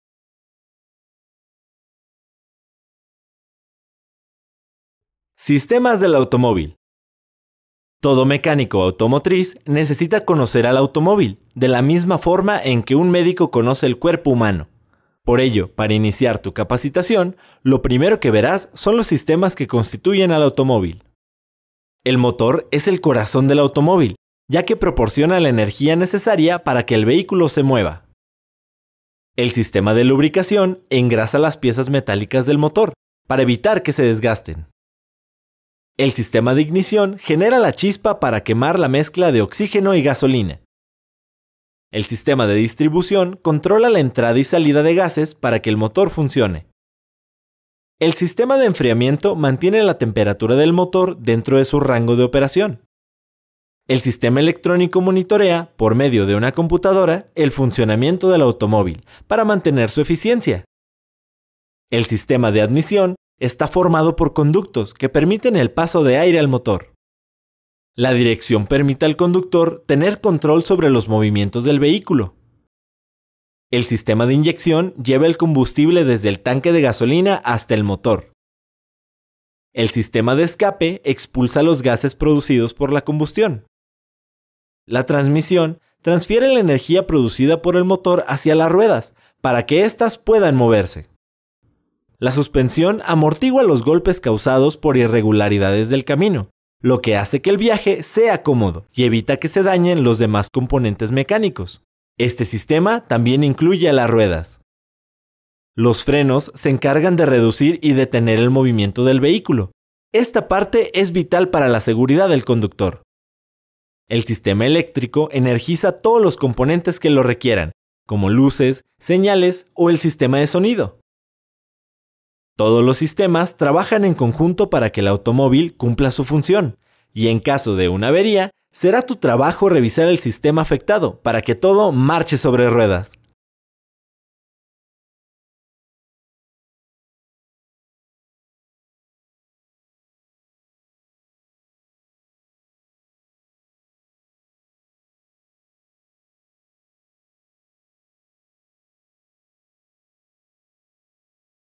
Sistemas del automóvil
Aprende todos los sistemas del automóvil, lo que te permitirá desarrollarte como mecánico automotriz.